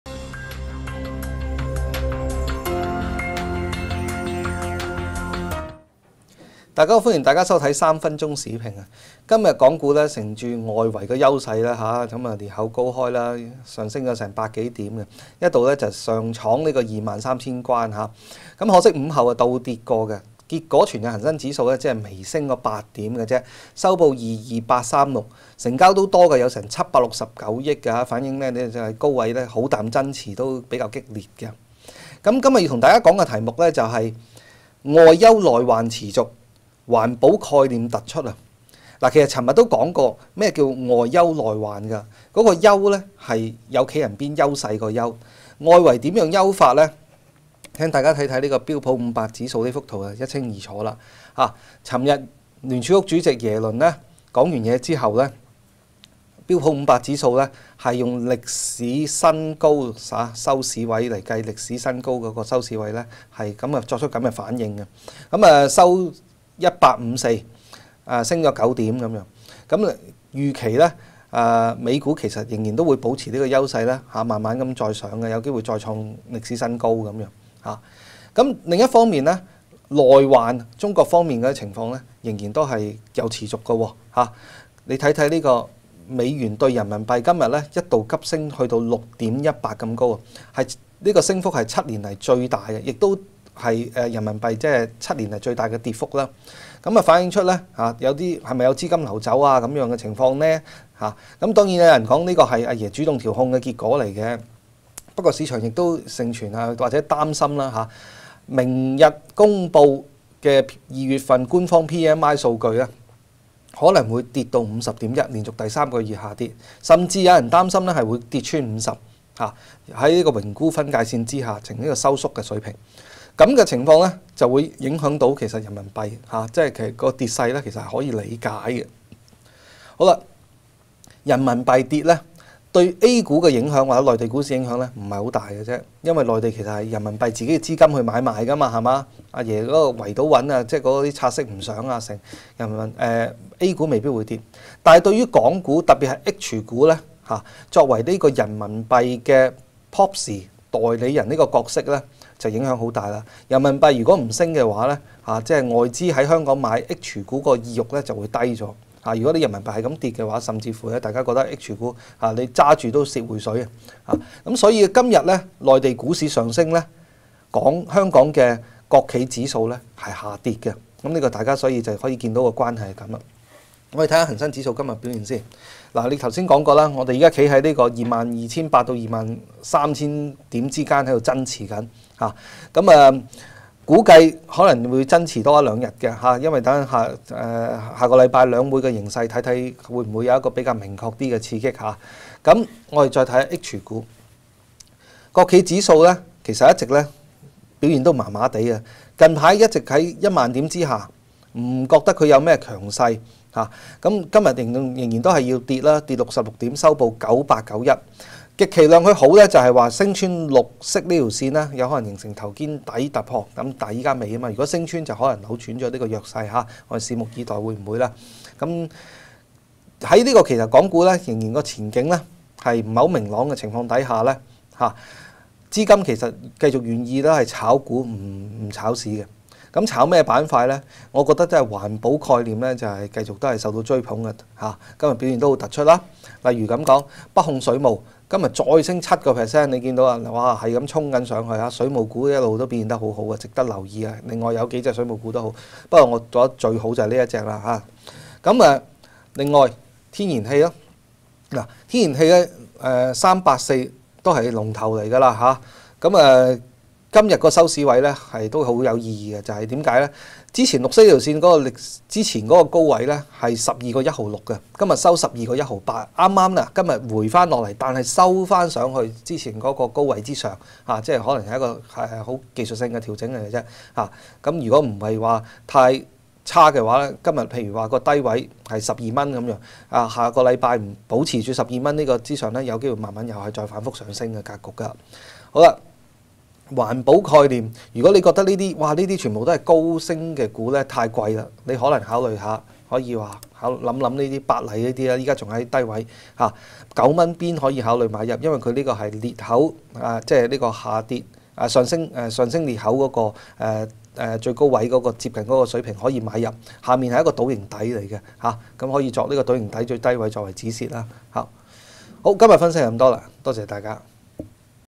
【三分鐘市評】李國慶︰外「優」內患持續 環保概念突出（2014年2月28日）
人民幣下跌，《投資理財》主編李國慶認為對內地股市影響不大，惟作為人民幣proxy的H股，影響很大. （更多內容﹕

【經濟日報】更多分析請訂閱hket.com，現提供14天免費試用